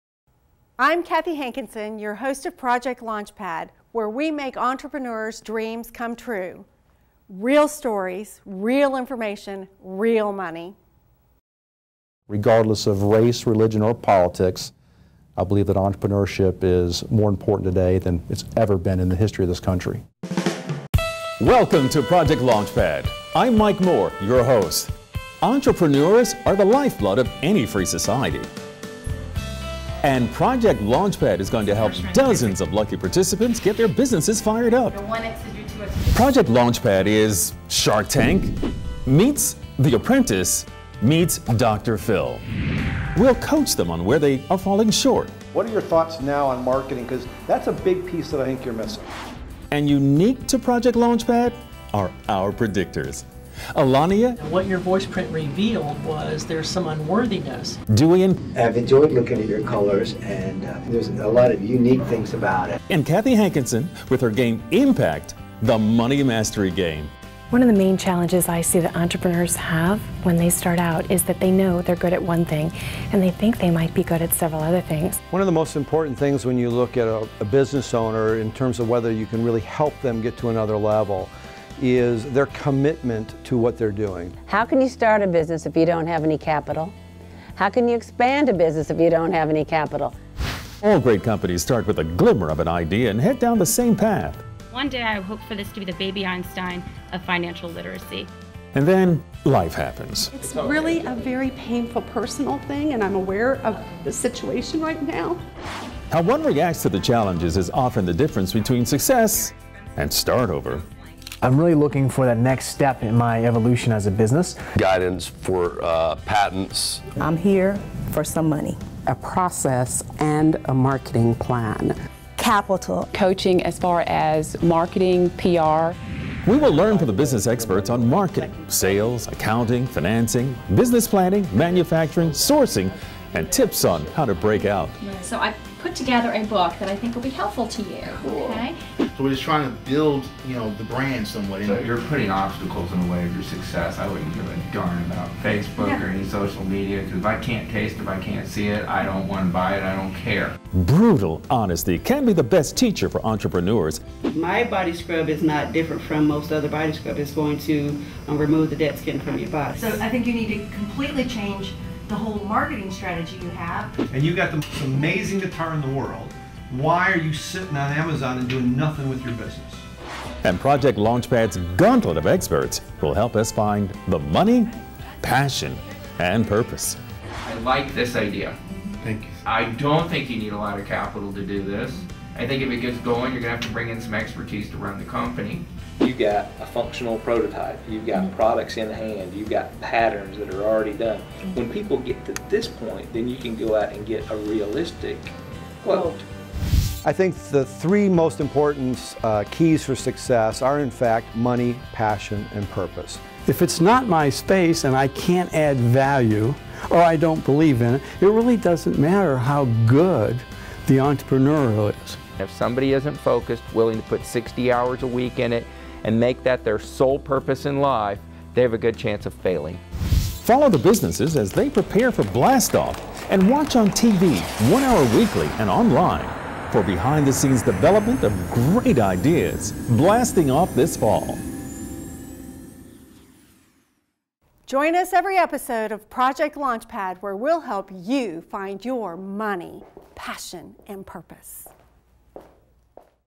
Project Launchpad sizzle reel promo
Project Launchpad sizzle reel. This is the promo reel to show TV networks, to demonstrate the concept of the show. This is not like a commercial for viewers, although has similarities. We'd love to hear what YOU think! Thank you for viewing!